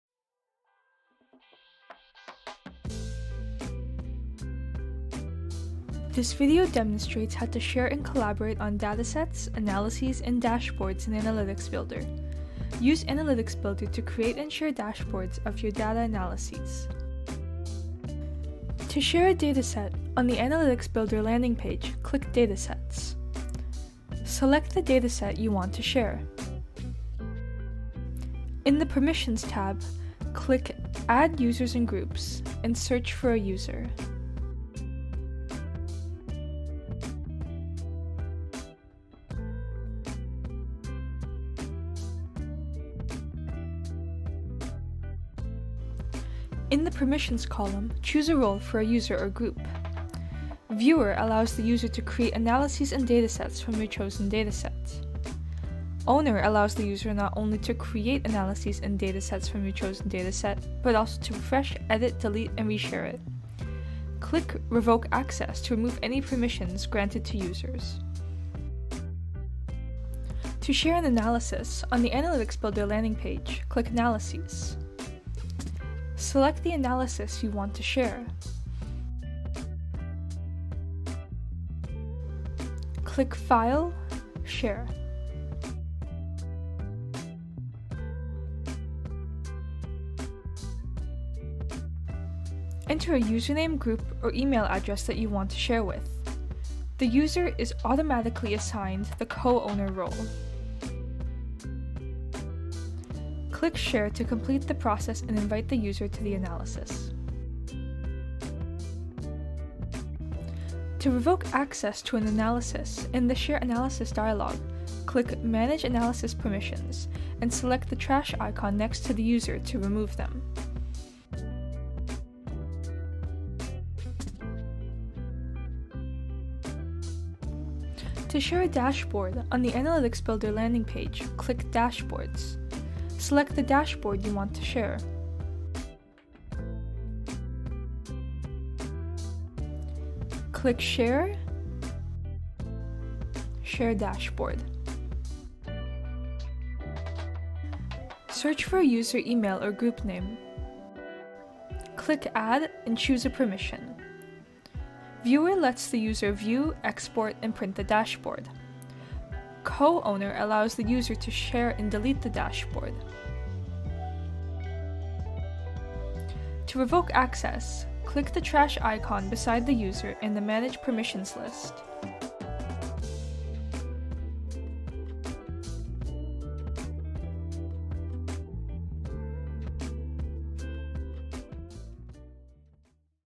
Sharing and collaborating with Analytics Builder | Administrator
This video demonstrates how to share and collaborate on datasets, analyses, and dashboards in Analytics Builder. Use Analytics Builder to create and share dashboards of your data analyses.